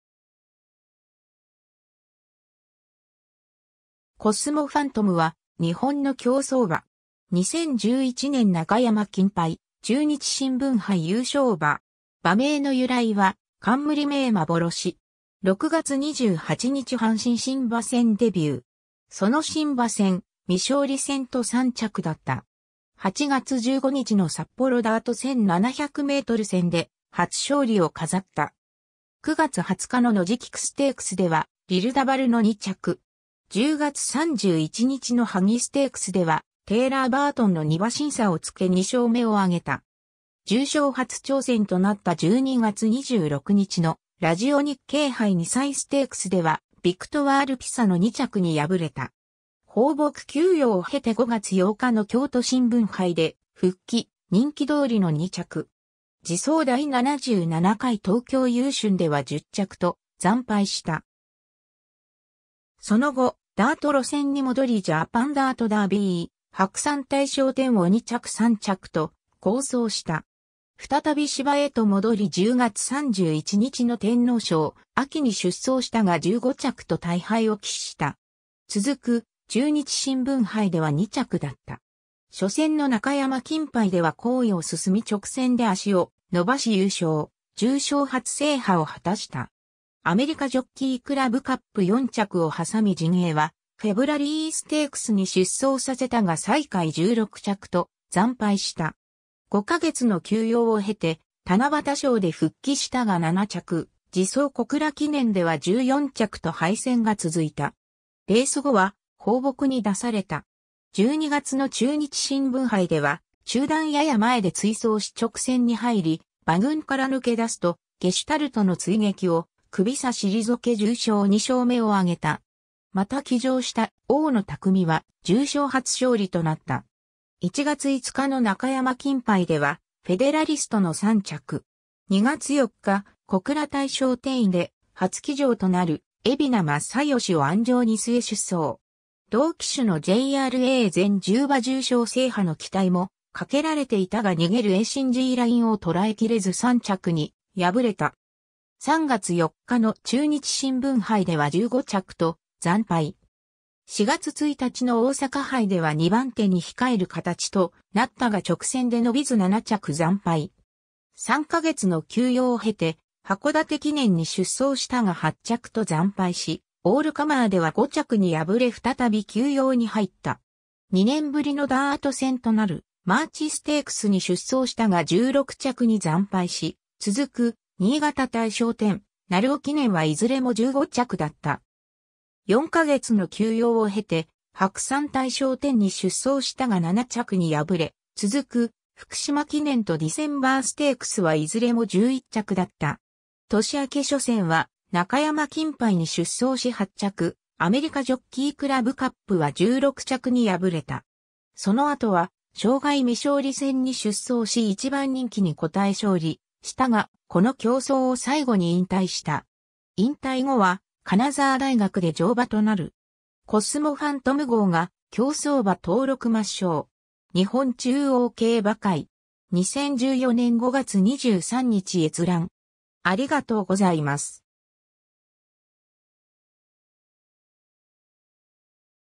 コスモファントム
コスモファントム (英: Cosmo Phantom) は日本の競走馬。
2011年中山金杯・中日新聞杯優勝馬。
馬名の由来は冠名+幻。
 6月28日阪神新馬戦デビュー。
その新馬戦・未勝利戦と3着だった。
8月15日の札幌ダート1700m戦で初勝利を飾った。
9月20日の野路菊ステークスではリルダヴァルの2着。
10月31日の萩ステークスではテイラーバートンの2馬身差をつけ2勝目を挙げた。
重賞初挑戦となった12月26日のラジオNIKKEI杯2歳ステークスではヴィクトワールピサの2着に敗れた。
 放牧休養を経て5月8日の京都新聞杯で復帰、人気通りの2着。
次走第77回東京優駿では10着と惨敗した。
その後ダート路線に戻りジャパンダートダービー・白山大賞典を2着3着と好走した。
再び芝へと戻り10月31日の天皇賞・秋に出走したが15着と大敗を喫した。
続く中日新聞杯では2着だった。
 初戦の中山金杯では好位を進み直線で脚を伸ばし優勝、重賞初制覇を果たした。
アメリカジョッキークラブカップ4着を挟み陣営はフェブラリーステークスに出走させたが最下位16着と惨敗した。
5ヶ月の休養を経て七夕賞で復帰したが7着、次走小倉記念では14着と敗戦が続いた。
レース後は放牧に出された。
12月の中日新聞杯では中団やや前で追走し直線に入り馬群から抜け出すとゲシュタルトの追撃をクビ差退け重賞2勝目を挙げた。
また騎乗した大野拓弥は重賞初勝利となった。
 1月5日の中山金杯ではフェデラリストの3着。
2月4日小倉大賞典で初騎乗となる蛯名正義を鞍上に据え出走。
同騎手のJRA全10場重賞制覇の期待もかけられていたが逃げるエーシンジーラインを捕らえきれず3着に敗れた。
3月4日の中日新聞杯では15着と惨敗。
4月1日の大阪杯では2番手に控える形となったが直線で伸びず7着惨敗。
3ヶ月の休養を経て函館記念に出走したが8着と惨敗し、オールカマーでは5着に敗れ再び休養に入った。
 2年ぶりのダート戦となるマーチステークスに出走したが16着に惨敗し、続く新潟大賞典、鳴尾記念はいずれも15着だった。
4ヶ月の休養を経て白山大賞典に出走したが7着に敗れ、続く福島記念とディセンバーステークスはいずれも11着だった。
 年明け初戦は中山金杯に出走し8着、アメリカジョッキークラブカップは16着に敗れた。
その後は障害未勝利戦に出走し1番人気に応え勝利したが、この競走を最後に引退した。
 引退後は金沢大学で乗馬となる。
 ^ “コスモファントム号が競走馬登録抹消”. 日本中央競馬会 (2014年5月22日). 2014年5月23日閲覧。